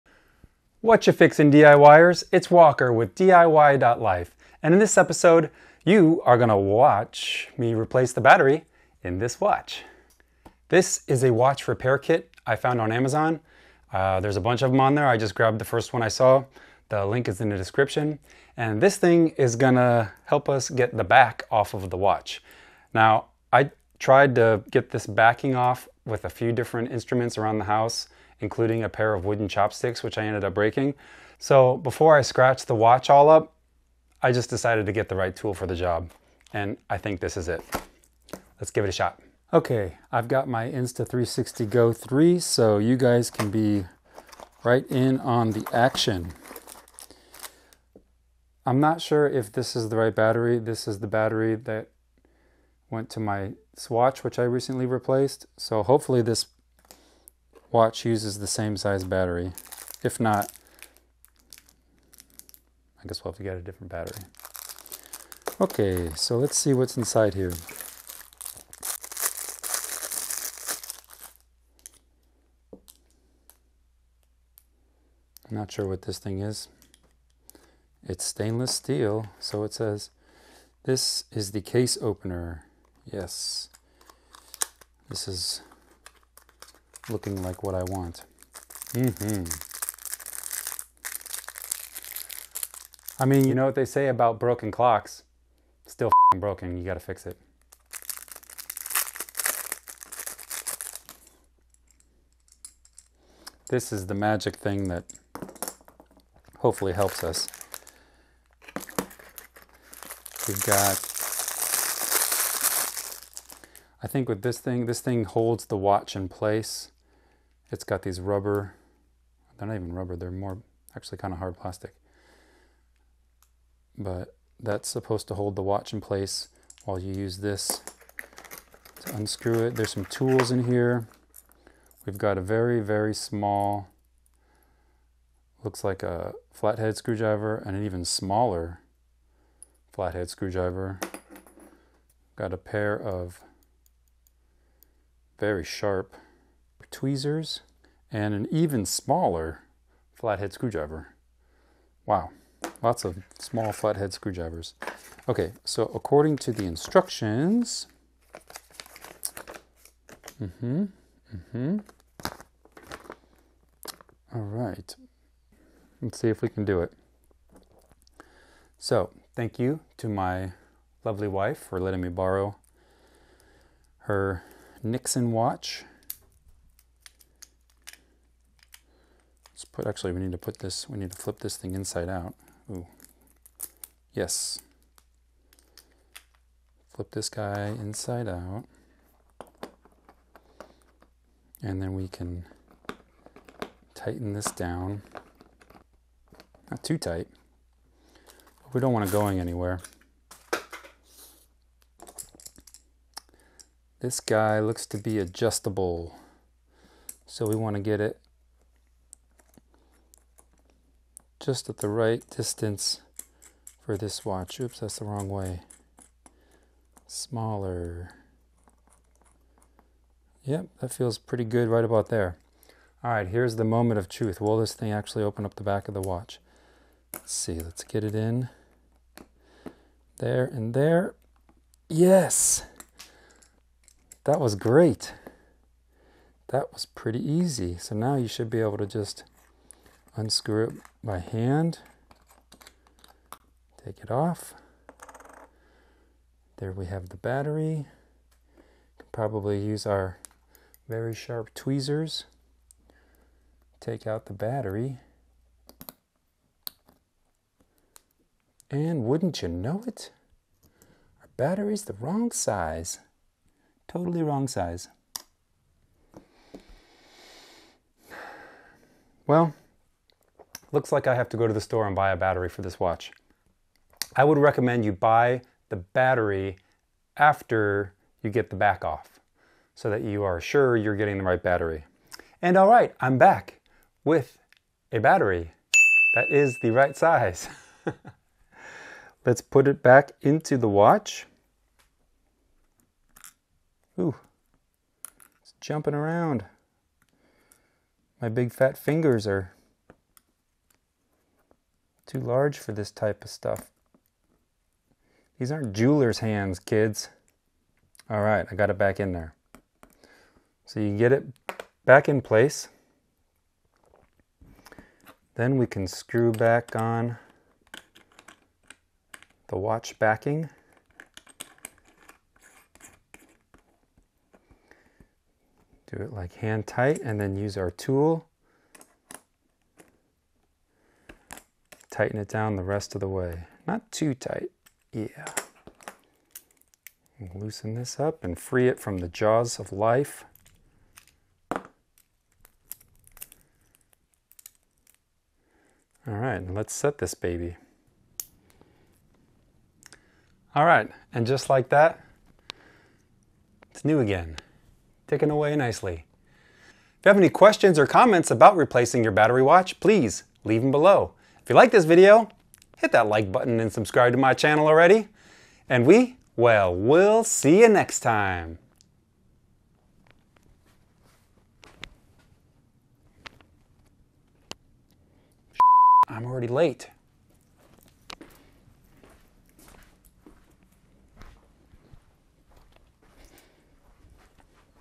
How to Replace Your Watch Battery
In this video I'll be replacing the battery in my wife's Nixon watch. This watch has a plate on the back that needs to be unscrewed and it's very hard to do without the right tool so I would highly recommend getting a watch repair kit off of Amazon and it should contain a watch back remover tool. The Nixon watch uses a battery type SR626SW. If you're not sure what size battery your watch has I would hold off on buying one until you remove the old battery and read the battery model right off the old battery. After not finding my battery at Walmart and Rite Aid I would recommend just getting your new batteries off Amazon. You have to buy like a 10 or 20 pack of them but they're so cheap, it's a no-brainer.
Watch Battery Replacement Steps
00:10 - Unboxing the Watch Replacement Kit
02:46 - Removing the Watch Back
04:06 - Removing the Old Battery
04:29 - Installing a New Battery
05 :12 - Replacing Watch Back
05:45 - Setting Watch Time
Shopping List
ToFree Watch Repair Kit